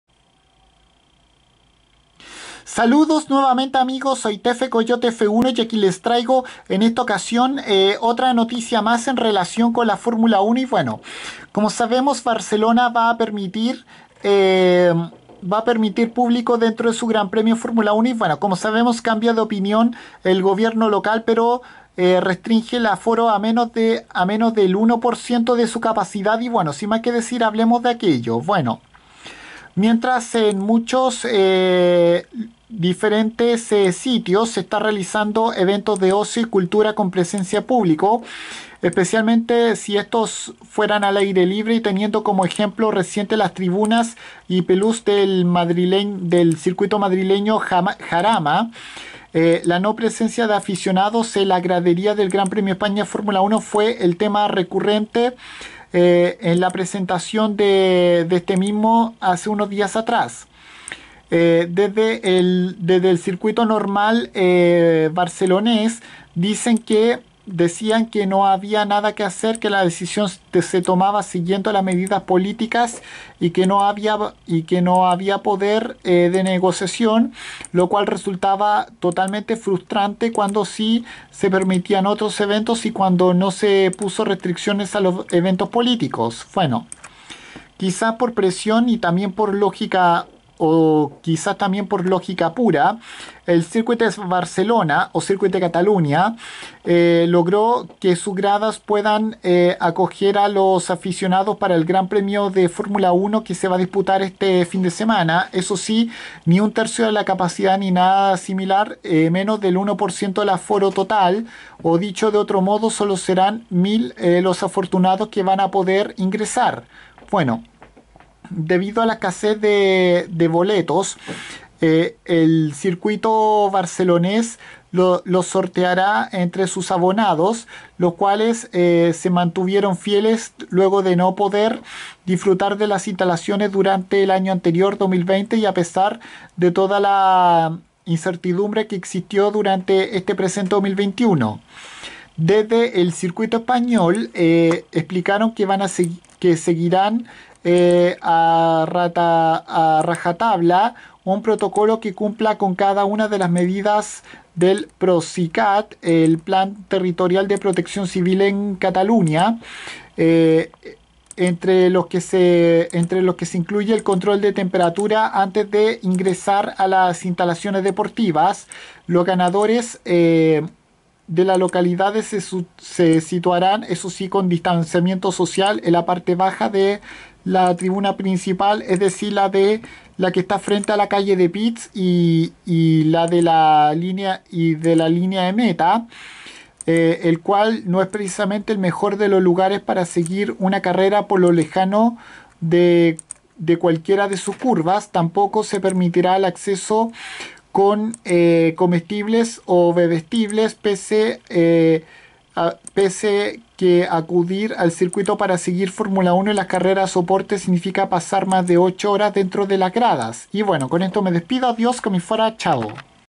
El Circuit de Catalunya de Barcelona permitirá público en su GP de F1
Solo tendrá un aforo del 1% de su capacidad para este fin de semana en el Gran Premio de España de Fórmula 1 en su versión 2021